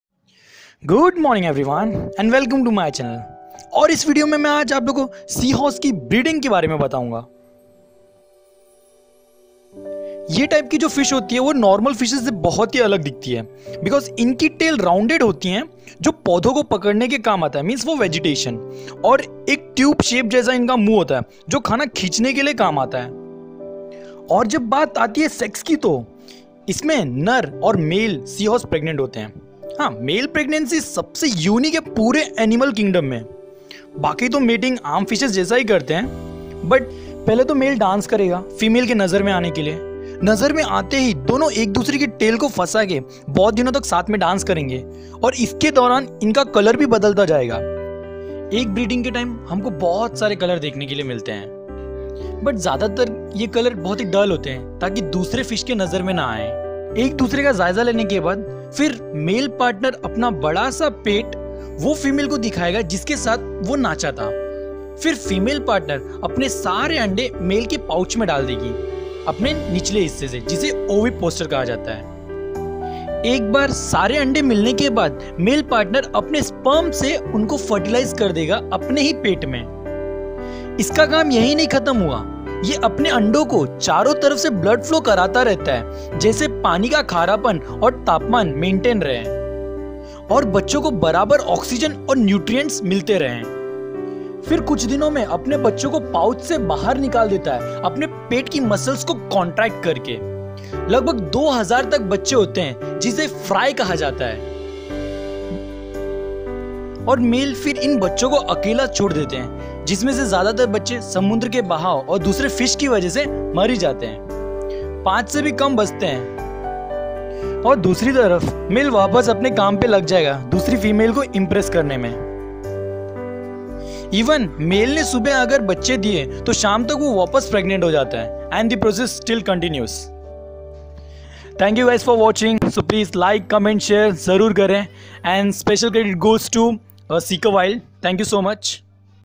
Male pregnancy video
SPECIAL CREDIT - SEEKER WILD

For male seahorses, parenting skills and being a sweet dancer are a must to make it in the underwater dating scene.